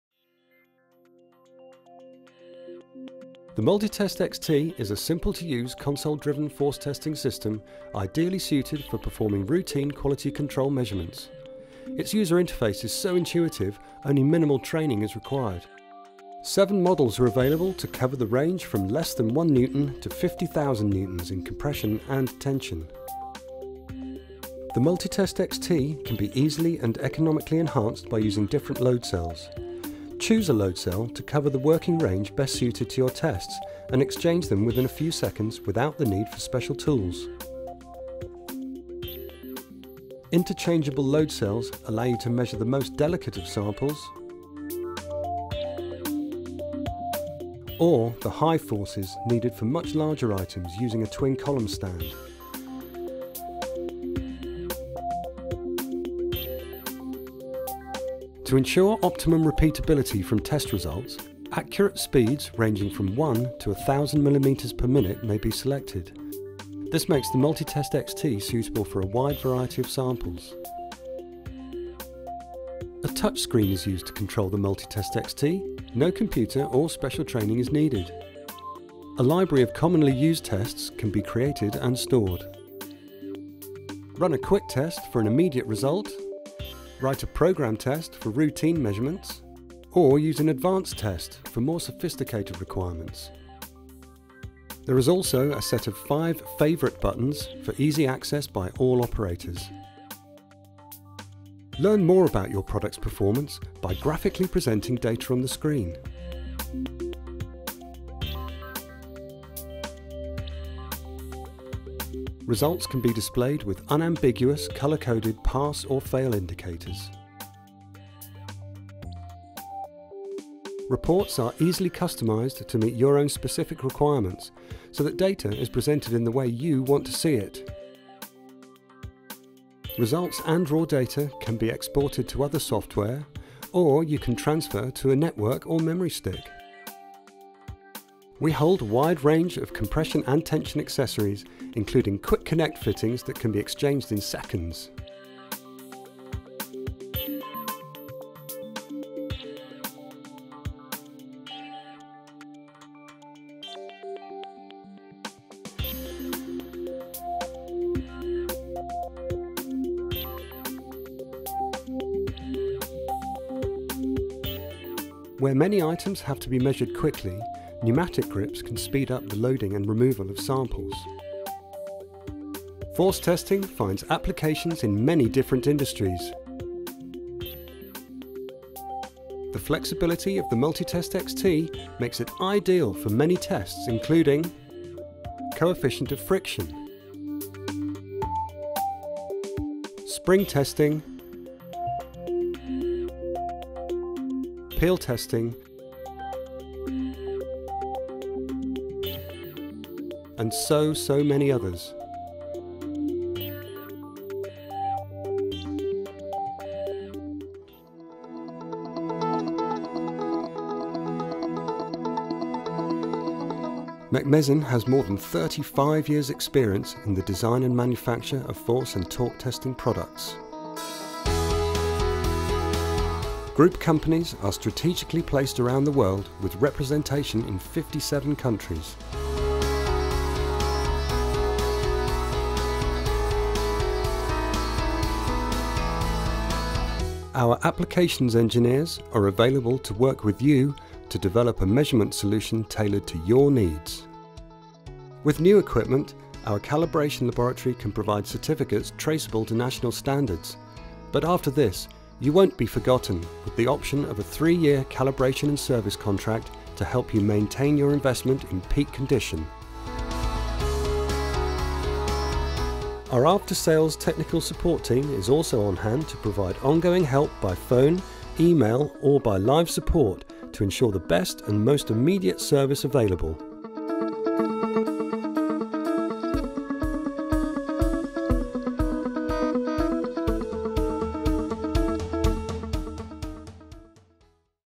MultiTest-xt Touch-screen Force Tester - Product Overview (In-depth) - Mecmesin Force Measurement
The MultiTest-xt range of easy to use force measuring systems are specifically designed for conducting tensile and compression tests within the manufacturing environment. Controlled by a built-in touch-screen console, only minimal training is necessary to begin testing and results are easy to interpret with a colour coded "Pass" or "Fail" indication.  Reports, including graphs if required, can be printed or saved.   The versatile MultiTest-xt can be used for a wide variety of test applications including coefficient of friction, flexure/bend testing, peel testing, spring testing, tear strength and top-load testing